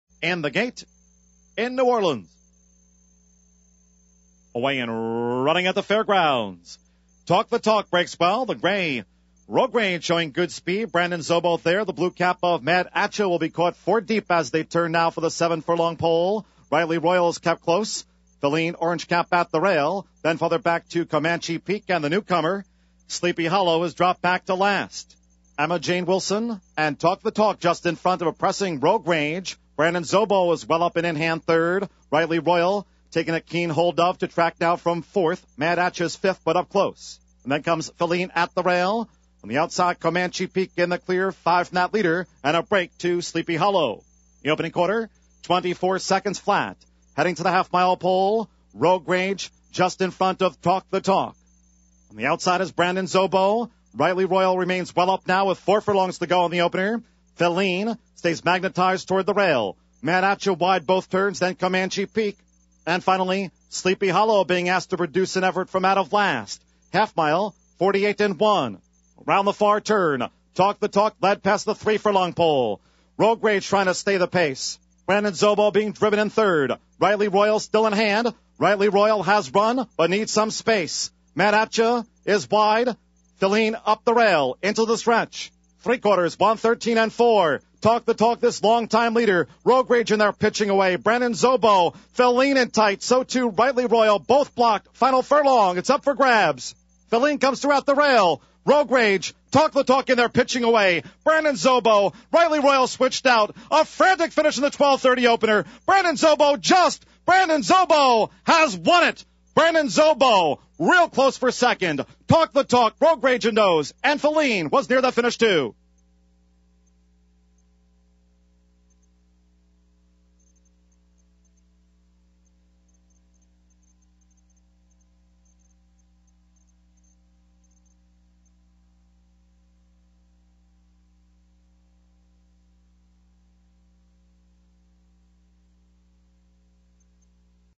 FAIR GROUNDS, 2007-02-09, Race 1
Entries: Thoroughbred Race, FAIR GROUNDS, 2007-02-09, Race 1:
      (Pgm-Runner-Jockey-Finish)
       1-Talk the Talk (KY), Wilson, E, finished 2;
       2-Filene (FL), Talamo, J, finished 4;
       3-Rightly Royal (KY), Albarado, R, finished 5;
       4-Rogue Rage (KY), Castanon, J, finished 3;
       5-Comanche Peak (TX), Campbell, J, finished 6;
       6-Sleepy Hollow (KY), Riquelme, J, finished 7;
       7-Brandon's Oboe (MD), Lanerie, C, finished 1;
       8-Mad At Cha (TX), Theriot, J, finished 8;